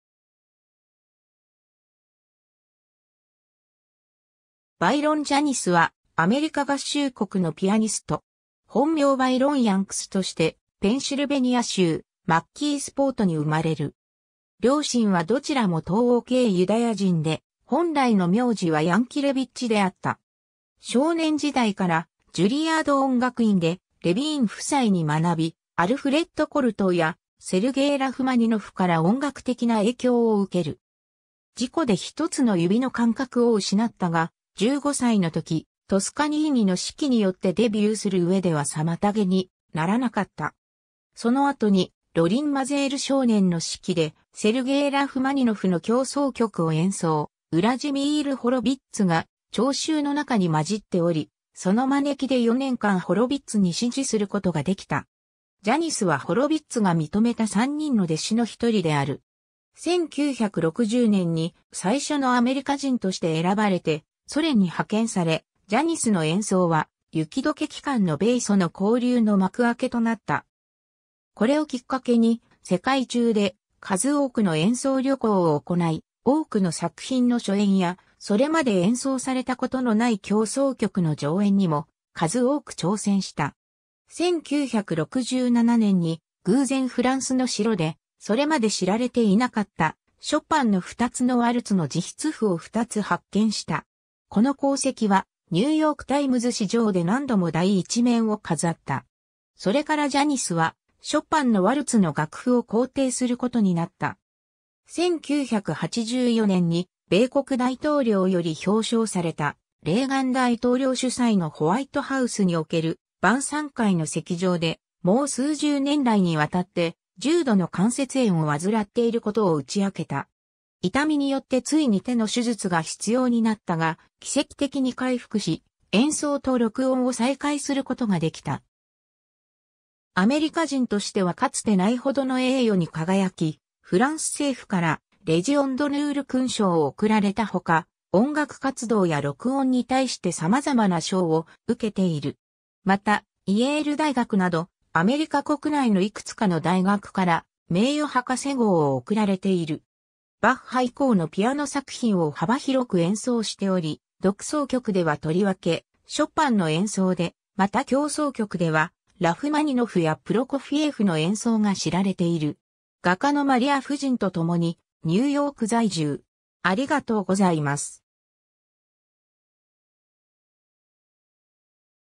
バイロン・ジャニス
バイロン・ジャニス（Byron Janis、1928年3月24日 - ）は、アメリカ合衆国のピアニスト。
 本名バイロン・ヤンクス（Byron Yanks）としてペンシルベニア州マッキースポートに生まれる。
両親はどちらも東欧系ユダヤ人で、本来の苗字はヤンキレヴィッチ（Yankilevich）であった。
少年時代からジュリアード音楽院でレヴィーン夫妻に学び、アルフレッド・コルトーやセルゲイ・ラフマニノフから音楽的な影響を受ける。
事故で一つの指の感覚を失ったが、15歳のときトスカニーニの指揮によってデビューする上では妨げにならなかった。
その後にロリン・マゼール少年の指揮でセルゲイ・ラフマニノフの協奏曲を演奏、ウラジミール・ホロヴィッツが聴衆の中に交じっており、その招きで4年間ホロヴィッツに師事することができた。
ジャニスはホロヴィッツが認めた3人の弟子のひとりである（他のふたりはロナルド・トゥリーニとゲイリー・グラフマン）。
 1960年に、最初のアメリカ人として選ばれてソ連に派遣され、ジャニスの演奏は雪融け期間の米ソの交流の幕開けとなった。
これをきっかけに世界中で数多くの演奏旅行を行い、多くの作品の初演や、それまで演奏されたことのない協奏曲の上演にも数多く挑戦した。
1967年に、偶然フランスの城で、それまで知られていなかったショパンの二つのワルツの自筆譜を二つ発見した。
この功績は、ニューヨーク・タイムズ紙上で何度も第一面を飾った。
それからジャニスはショパンのワルツの楽譜を校訂することになった。
 1984年に米国大統領より表彰された、レーガン大統領主催のホワイトハウスにおける晩餐会の席上で、もう数十年来にわたって重度の関節炎を患っていることを打ち明けた。
痛みによってついに手の手術が必要になったが、奇蹟的に回復し、演奏と録音を再開することができた。
 アメリカ人としてはかつてないほどの栄誉に輝き、フランス政府からレジオン・ドヌール勲章を贈られたほか、音楽活動や録音に対して様々な賞を受けている。
また、イェール大学などアメリカ国内のいくつかの大学から名誉博士号を贈られている。
 バッハ以降のピアノ作品を幅広く演奏しており、独奏曲ではとりわけショパンの演奏で、また協奏曲ではラフマニノフやプロコフィエフの演奏が知られている。
 画家のマリア夫人（ゲイリー・クーパーの一人娘）とともにニューヨーク在住。